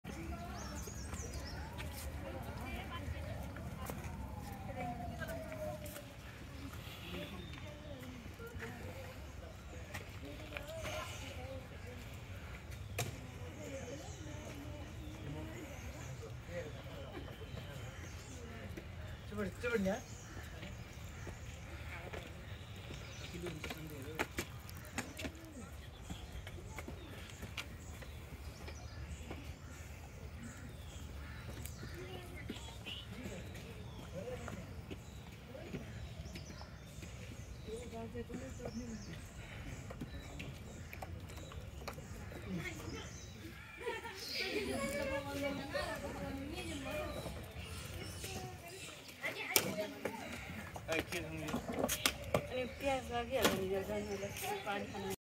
Monkeys near the Swayambhunath Stupa (Monkey Temple) in Kathmandu, Nepal
As I began my walk up the 365 steps to the Swayambhunath Stupa (Monkey Temple) in Kathmandu, I stopped to film some thirsty monkeys just monkeying around.